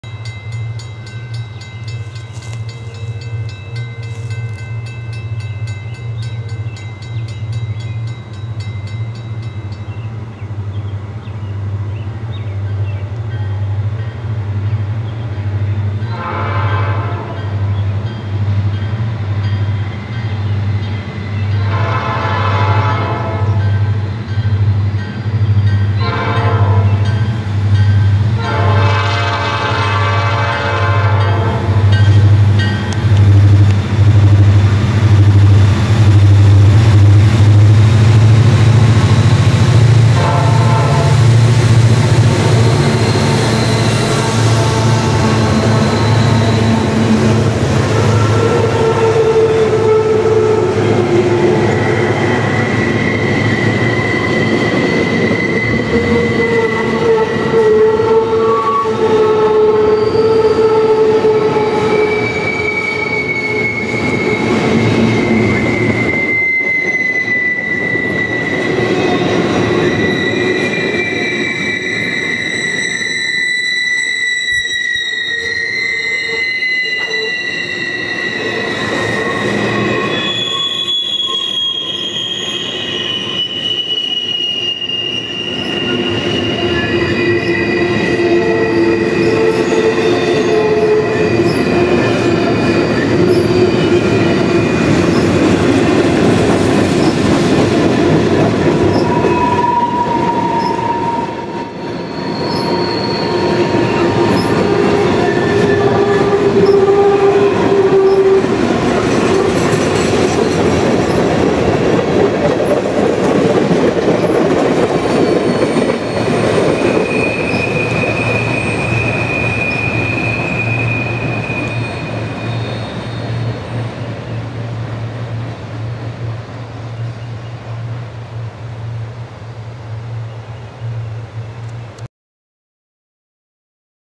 CP Train 159 on Laflin Hill
We Catch The Early 159 uphill as he is throttled up with DME 6085 leading.